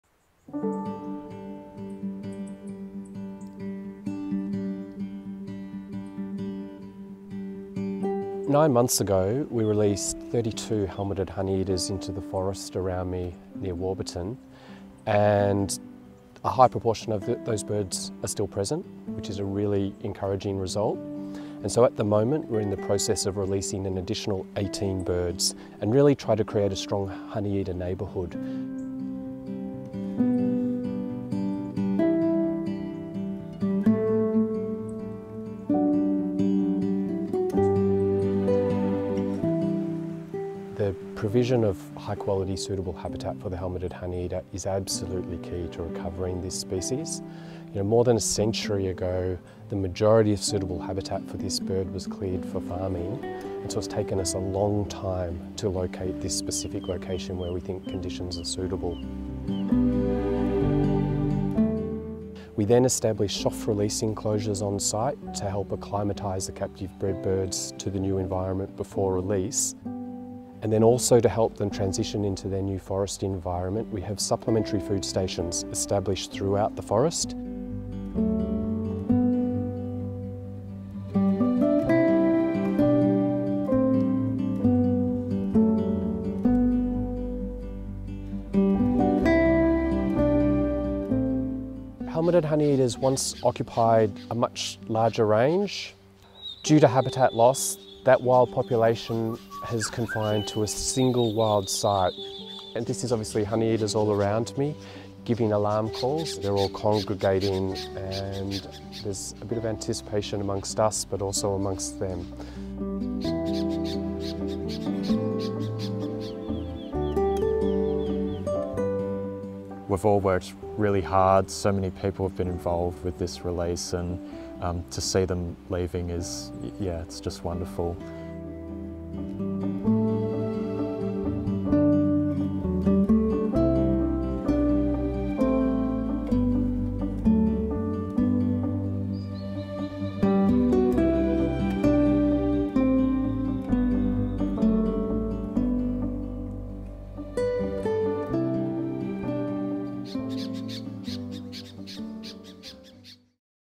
Helmeted Honeyeaters flying free in their new forest home
18 critically endangered Helmeted Honeyeaters have moved into their new forest home within Yarra Ranges National Park joining 32 birds that were released at this new site last year.
The Helmeted Honeyeater Recovery team includes the Department of Environment, Land, Water and Planning, Zoos Victoria, Parks Victoria, Friends of the Helmeted Honeyeater, Port Phillip and Westernport CMA, Melbourne Water, Trust for Nature, Birdlife Australia, Greening Australia, Royal Botanic Gardens, local councils and universities.

Funding is also provided by the Australian Government, the Victorian Government’s Faunal Emblem Program, The Merrin Foundation, and The Myer Foundation.